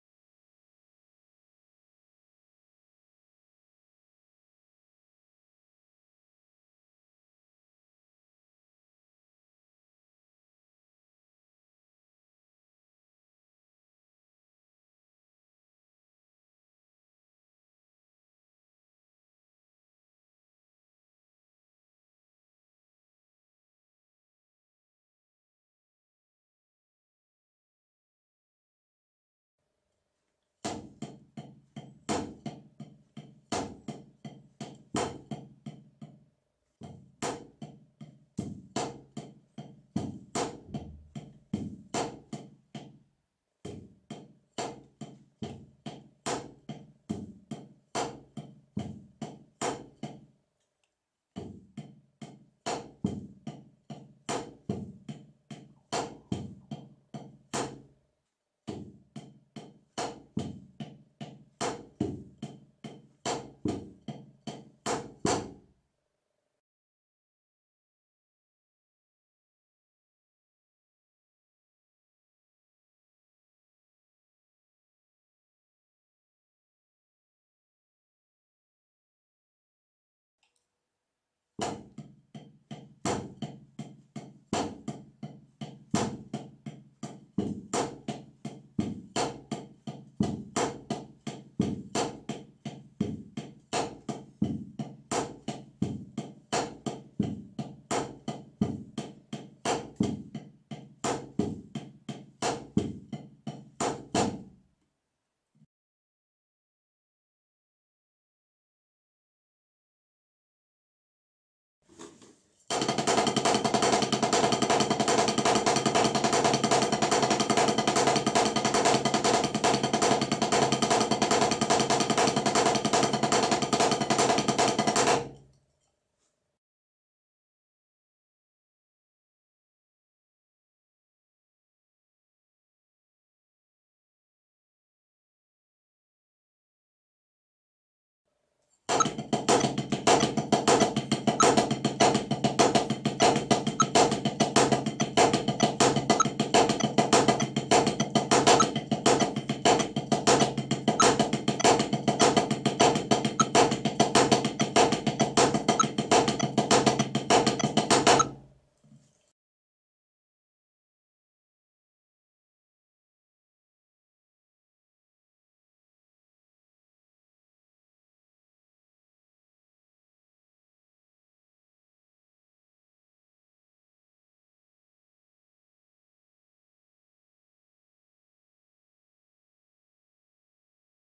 Paradiddle inversions accent mix up 2 - Drum lesson
Hey guys, I got a another cool paradiddle inversion accent  exercise for you. This is the varation of the other one i did a while agao.